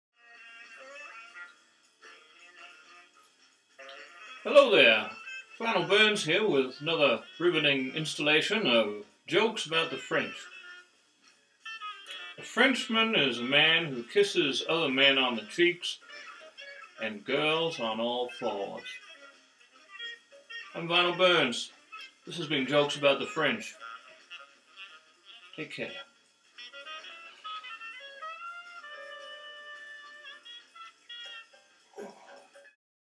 Vinyl Burns - Jokes About The French: Episode 2
Vinyl Burns reads jokes about the French, from the Serious Book Of One Liners by George Coote.

For Vinyl's own material, which, to be frank, will interest you a little more...
Here's more great Vinyl Burns stuff that you'll probably enjoy.